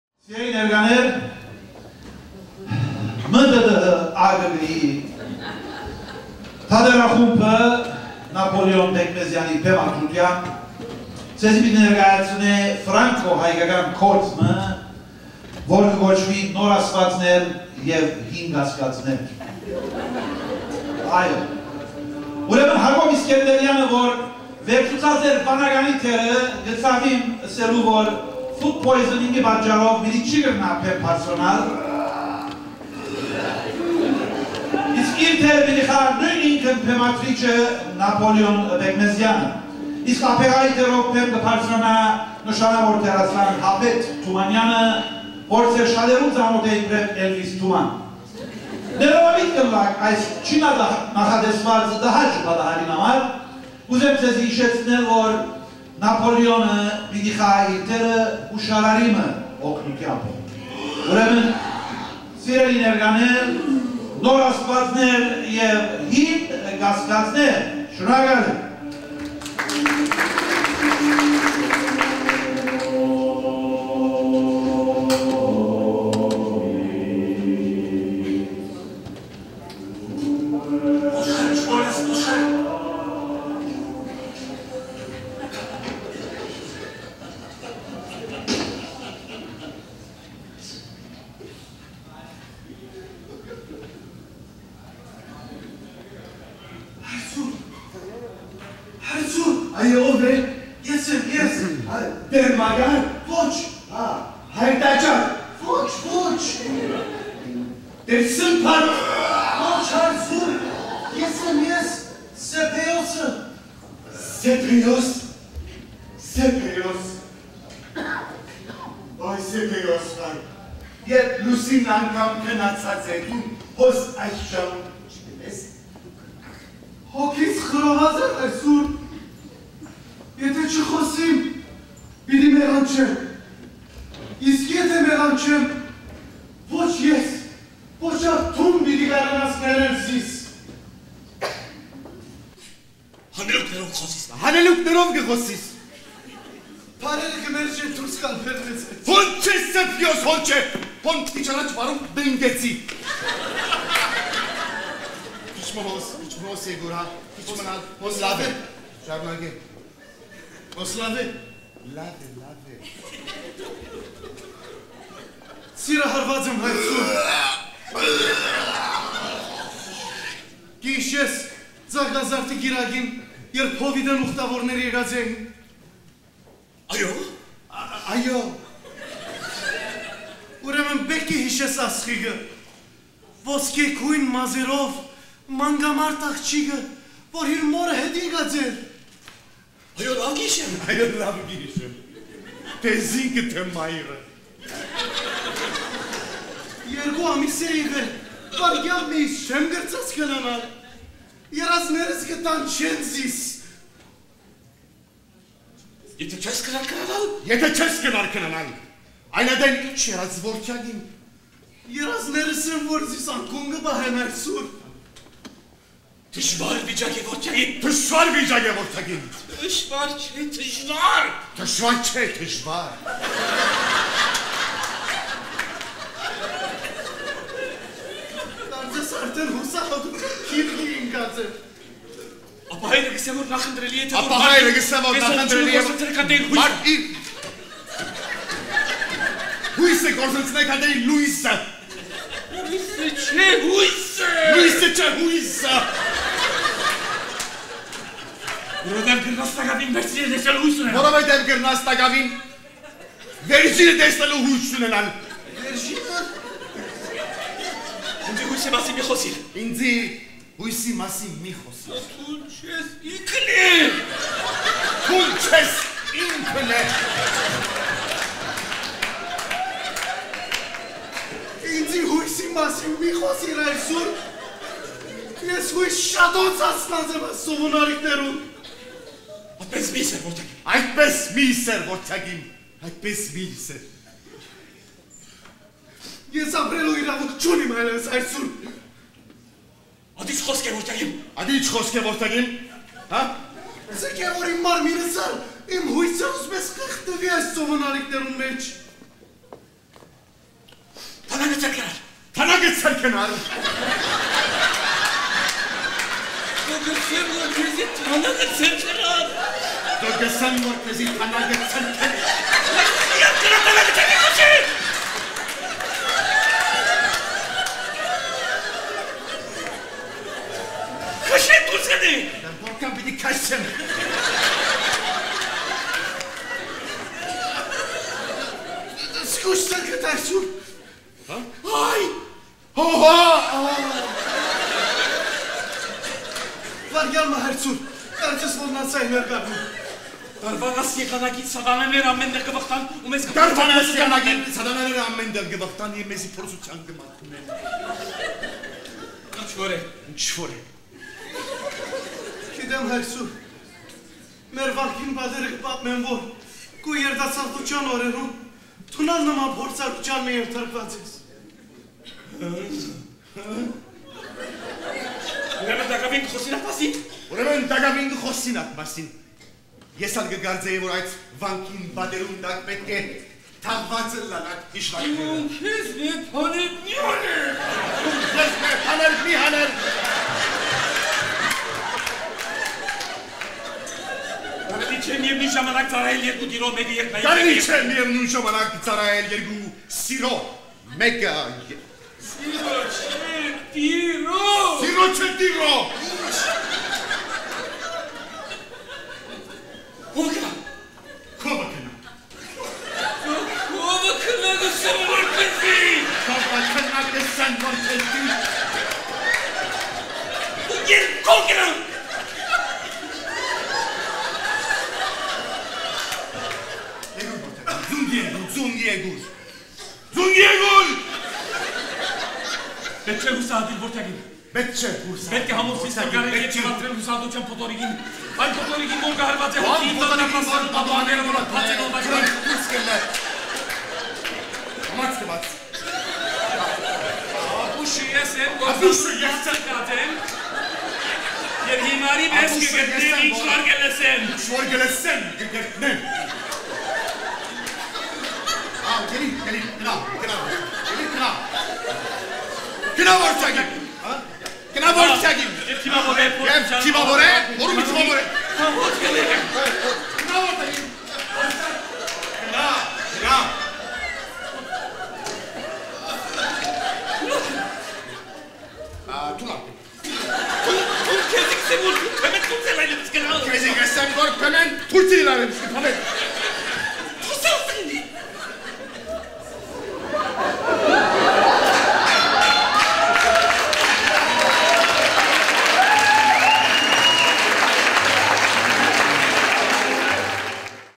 Nor Asdvadzner Hin Gasgadzner - Out Of The Cage | Vahe Berberian
OUT OF THE CAGE is a collection of short comedy skits and sketches, co-written by Vatchig Ter Sarkissian, Ara Madzounian and Henrik Mansourian.
Premiered in Los Angeles, in 2009, by Arvest. Directed by Vahe Berberian. Produced by Henrik Mansourian.